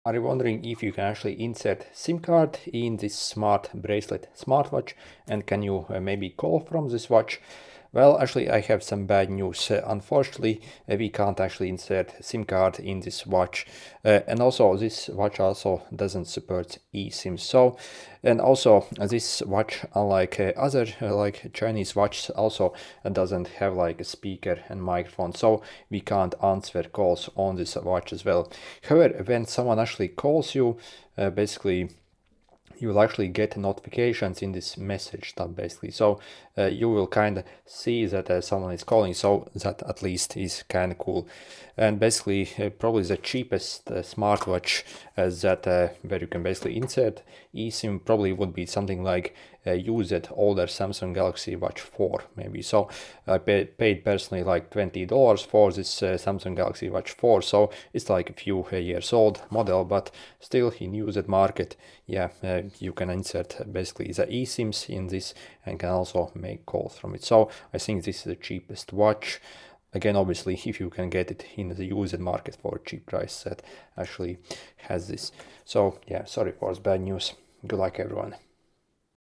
Can you Insert SIM Card in Smart Bracelet watch or ESIM? Can you Call & Receive Calls?
SIM & eSIM in  Smart Bracelet SmartWatch